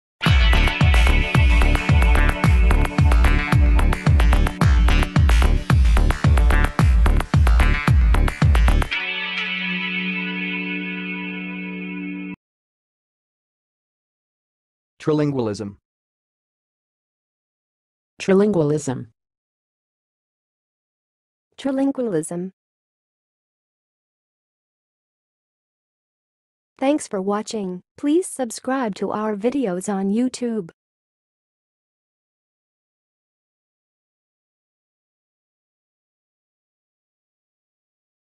How to say "trilingualism"! (High Quality Voices)
Watch in this video how to say and pronounce "trilingualism"!